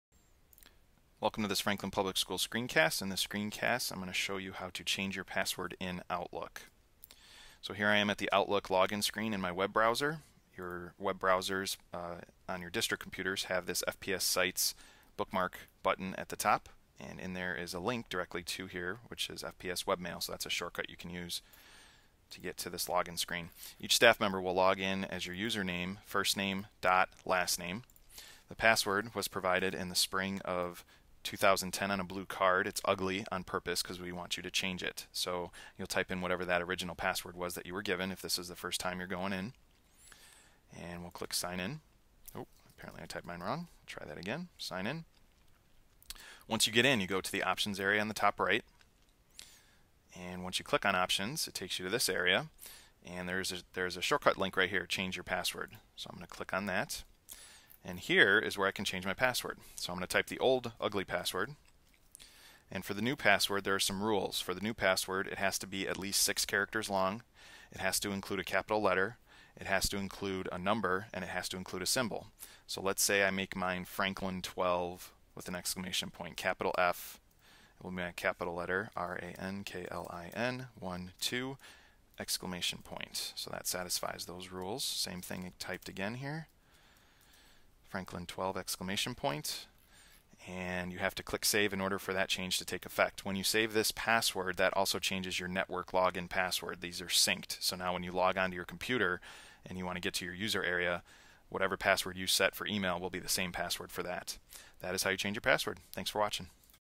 OutlookWebApp - Changing Your Password
This video shows Franklin Public Schools staff how to change their password in the Outlook Web App.